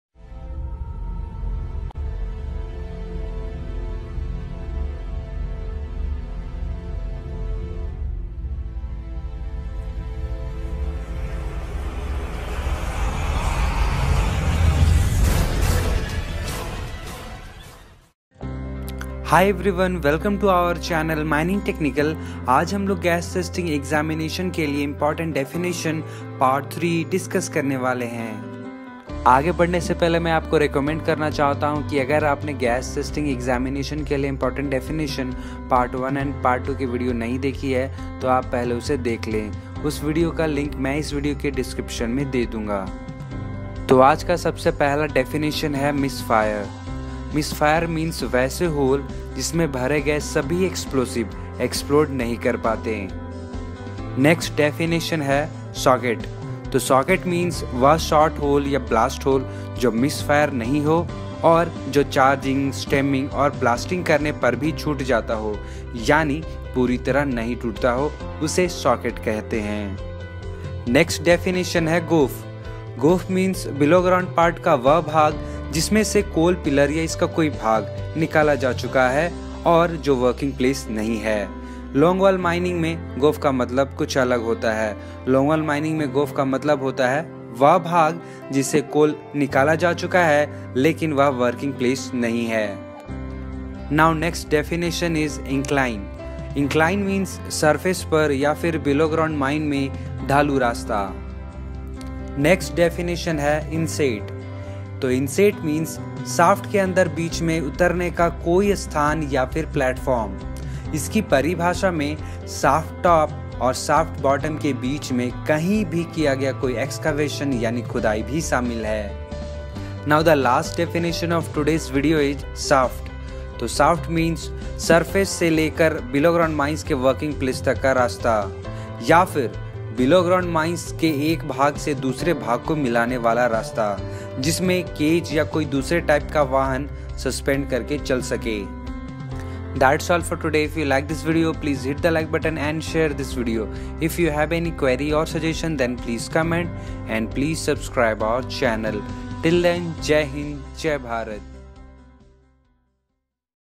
Important definition for gas testing examination || part 3 || mining examination || mining videos
Important definition for Gas Testing examination;

Contents – goaf, inset,incline socket,misfire,shaft